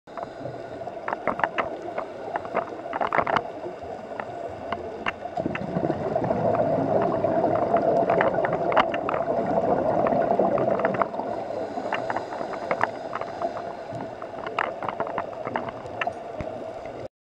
石垣島ダイビング【大崎ハナゴイリーフのウミガメ】
なぜか3ダイブともウミガメと遭遇！
まずは1ダイブ目、大崎ハナゴイリーフのウミガメ。
先客に追い出されてました。笑